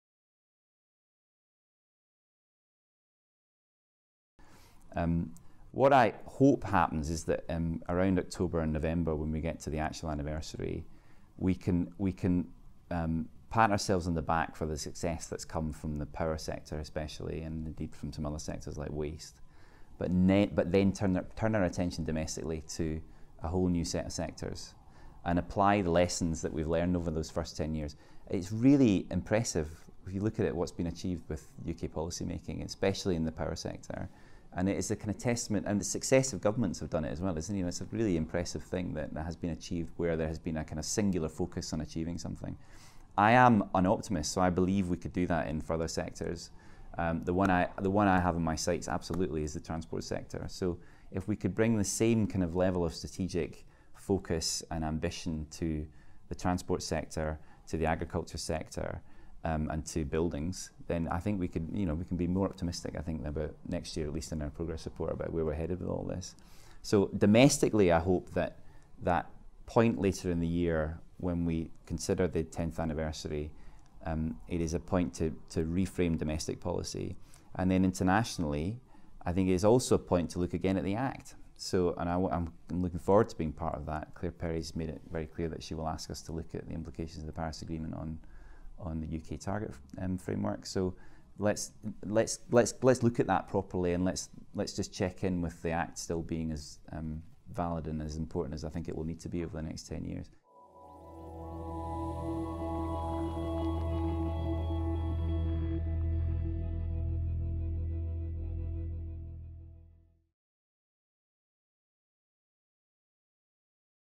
Chris Stark on the 10th anniversary of the Climate Change Act
The Carbon Brief Interview: Chris Stark, Cheif Executive, Committee on Climate Change

Carbon Brief: Regarding the Climate Change Act, and with the 10th anniversary this year of the act becoming law, what do you think would be a fitting way for the UK to mark that anniversary?

Chris Stark: "What I hope happens is that around October and November when we get to the actual anniversary, we can pat ourselves on the back for the success that's come from the power sector especially – and, indeed, from some other sectors like waste – but then turn our attention domestically to a whole new set of sectors and apply the lessons that we've learned over those first 10 years.

"It's really impressive, if you look at it, what's been achieved with UK policymaking, especially in the power sector. And successive governments have done it as well. It's a really impressive thing that has been achieved where there has been a kind of singular focus on achieving something. I am an optimist, so I believe we could do that in further sectors. The one I have in my sights absolutely is the transport sector. So if we could bring the same kind of level of strategic focus and ambition to the transport sector, to the agriculture sector, and to buildings, then I think we can be more optimistic about next year at least in our progress report, about where we're headed with all this.

"So, domestically, I hope that point later in the year when we consider the 10th anniversary, it is a point to reframe domestic policy and then, internationally, I think it's also a point to look again at the act. And I'm looking forward to being part of that. Claire Perry's made it very clear that she will ask us to look at the implications of the Paris Agreement on the UK target framework, so let's look at that properly and let's just check in with the act still being as valid and as important as I think it will need to be over the next 10 years."

The interview was conducted by Dr Simon Evans at the Committee on Climate Change's offices in London on 6 July 2018.